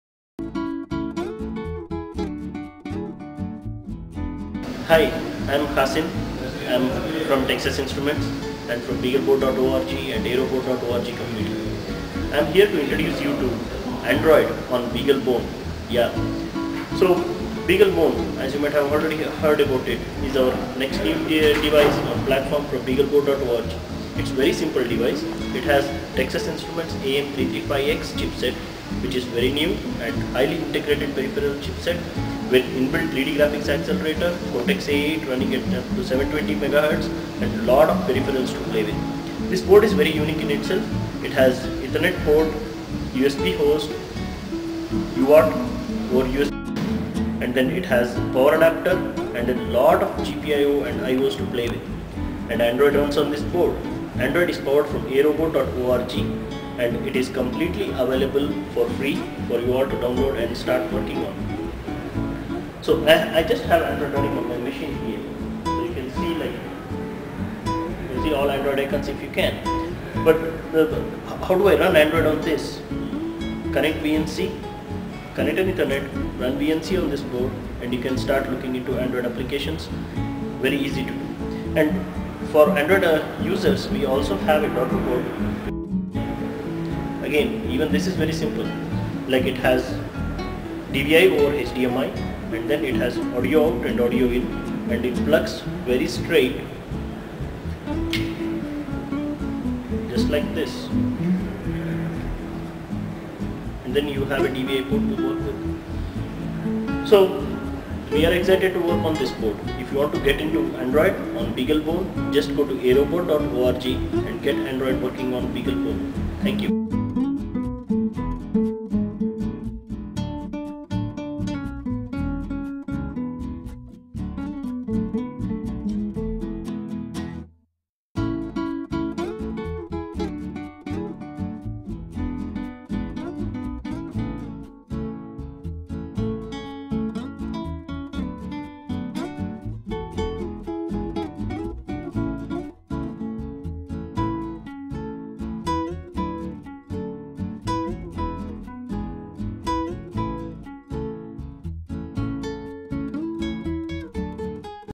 Android on TI AM335x Beaglebone
Khasim Introduces Android on Texas Instruments AM335x based Beaglebone at DroidCon.in